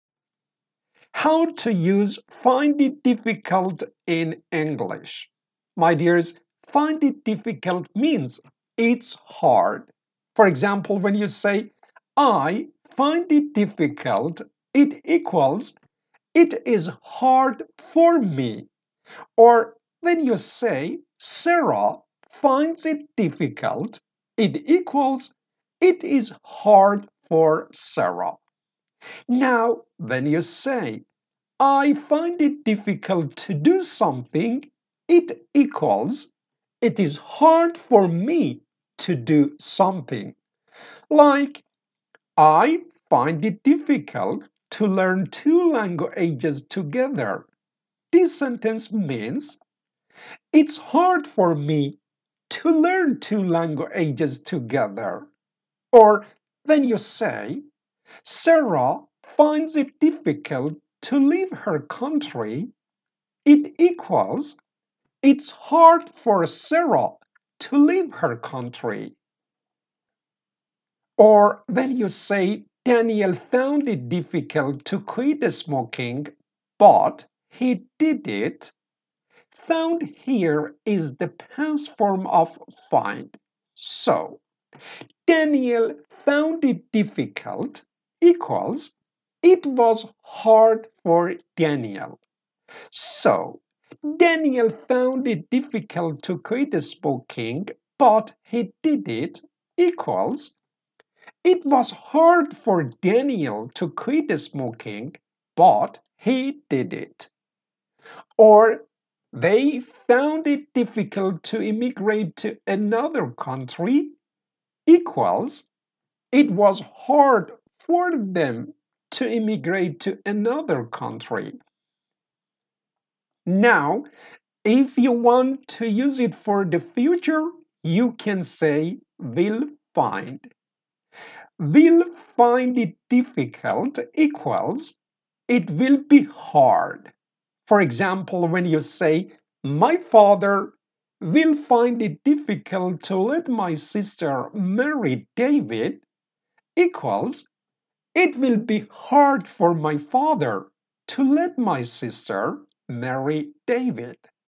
The meaning and Using "find it difficult"
My dears
find it difficult means it`s hard
When you say:
I find it difficult ...
it equals
it is hard for me ...
or when you say:
Sara finds it difficult
it equals
it`s hard for Sara ...
now
when you say:
I find it difficult to do something it means it`s hard for me to do something .
I find it difficult to learn two languages together.
 This sentence means
It`s hard for me to learn two languages together.
Sara finds it difficult to leave her country.
it means
it`s hard for Sara to leave her country.

Daniel found it difficult to quit smoking, but he did it.
found is the past form of find.
So Daniel found it difficult equals
It was hard for Daniel
so
equals
It was hard for Daniel to quit smoking, but he did it.
or
They found it difficult to immigrate to another country.
means
it was hard for them to immigrate to another country.
and
if you want to use it for the future, you can say.
My father will find it difficult to let my sister marry David.
this sentence means
it will be hard for my father to let my sister marry David.
now it`s your turn to tell me

What do you find difficult to do?
What did you find difficult to do?
What will you find difficult to do?